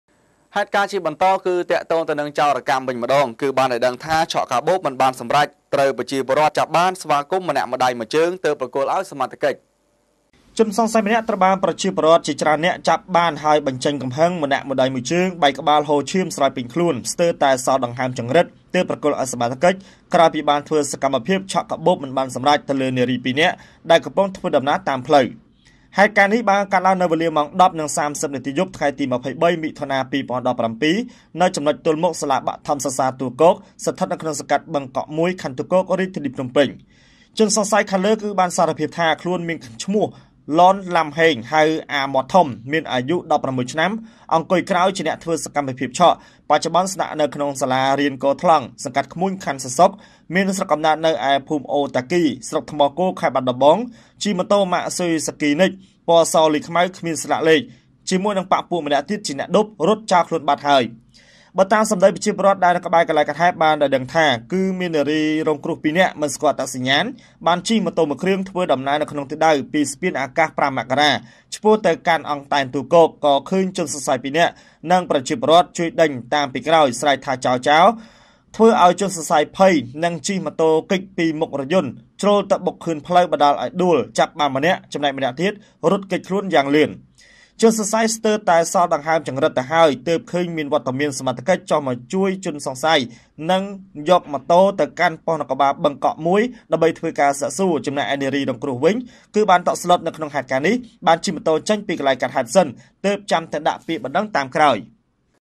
ឆក់កាបូបមិនបានសម្រេច ត្រូវប្រជាពលរដ្ឋចាប់បាន ស្វាគមន៍មួយដៃមួយ
ឆក់កាបូបមិនបានសម្រេច ត្រូវប្រជាពលរដ្ឋចាប់បាន ស្វាគមន៍មួយដៃមួយជើង ទើបប្រគល់ឲ្យសមត្ថកិច្ច